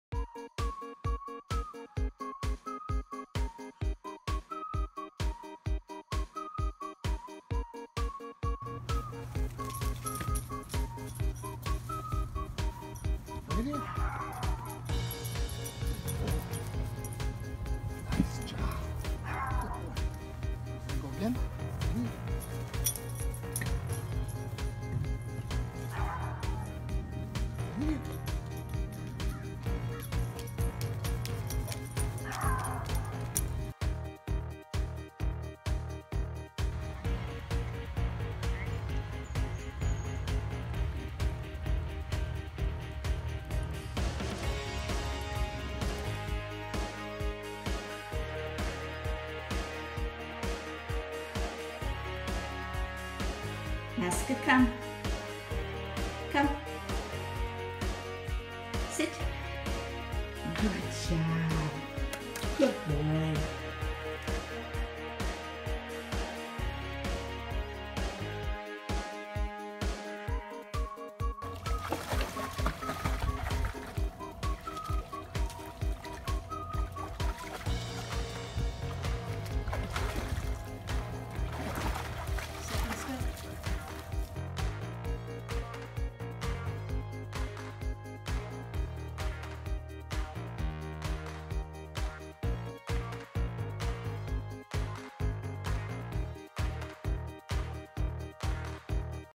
Laska - Foster Dog v.2
Our foster dog Laska. DFW Lab Rescue will be available in mid Feb 2019. Added footage.